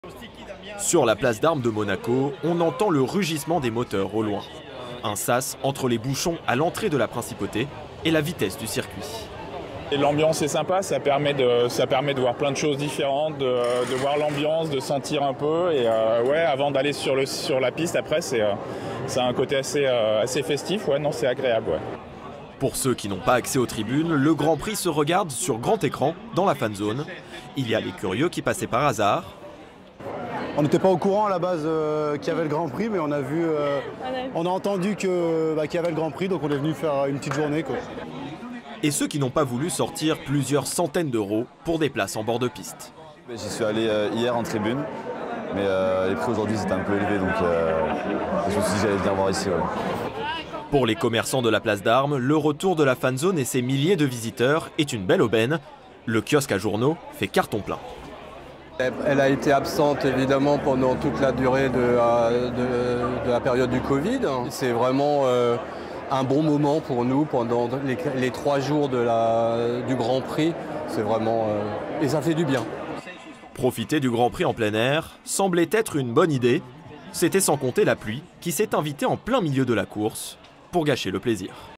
À Monaco, la fan zone du Grand Prix de F1 est de retour
Le Grand Prix de Monaco 2023 a fait renaître sa fan zone. Privés de spectacle depuis les confinements, les amoureux de voitures de course ont apprécié le retour de cet espace réservé à ceux qui n'ont pas forcément les moyens de se payer une place au bord du circuit.